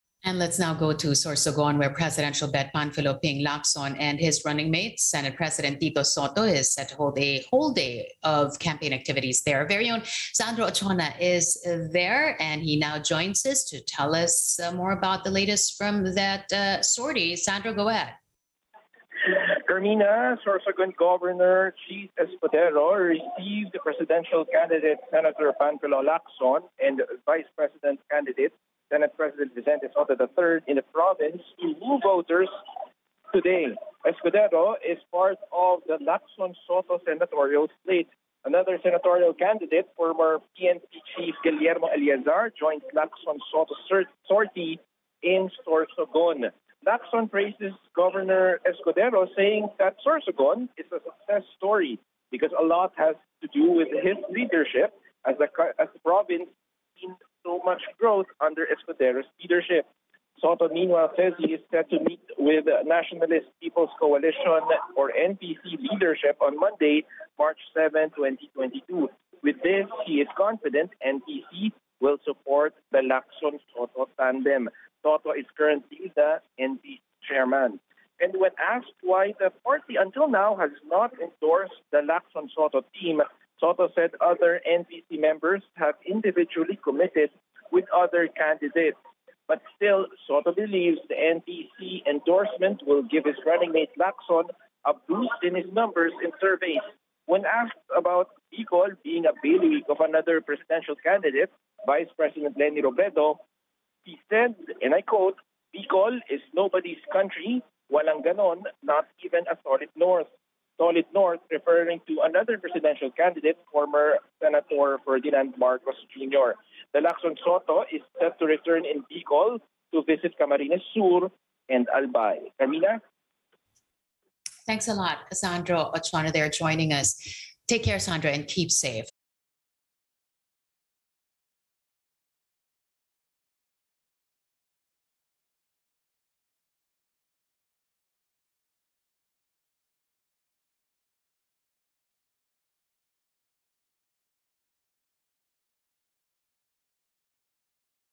HALALAN2022: Lacson-Sotto tandem campaigns in Sorsogon | AN
Dateline Philippines:  Presidential bet Panfilo 'Ping' Lacson and his runningmate, Senate President Tito Sotto are in Sorosogon to hold a whole day of campaign activities.